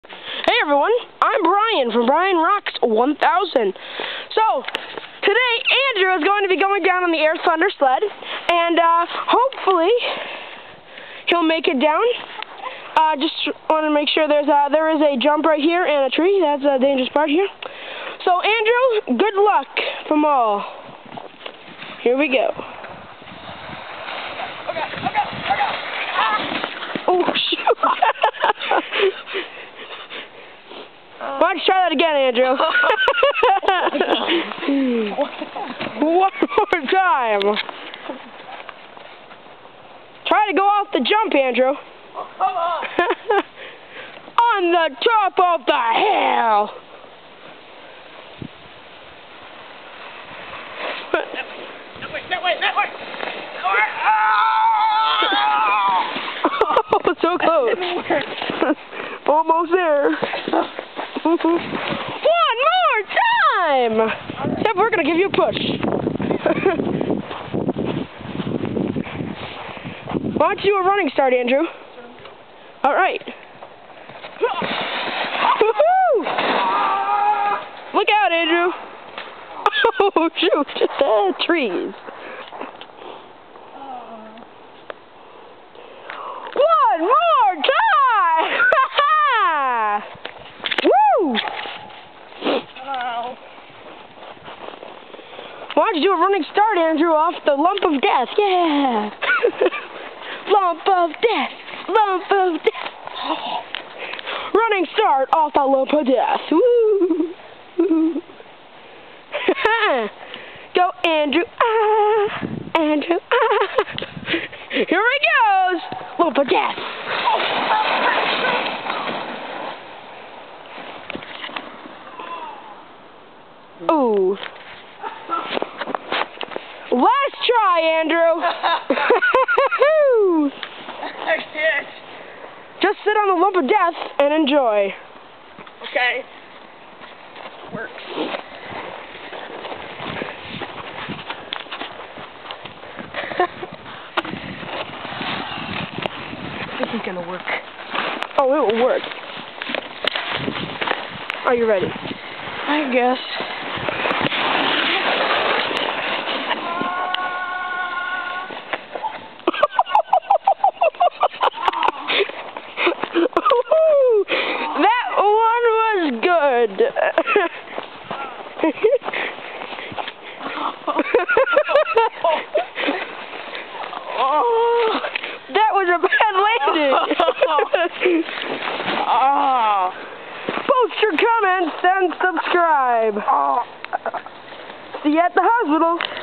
Wipeout Sledding
andrew goes down our extremely dangerous yet fun hill and crashes into a couple sticks and it was cool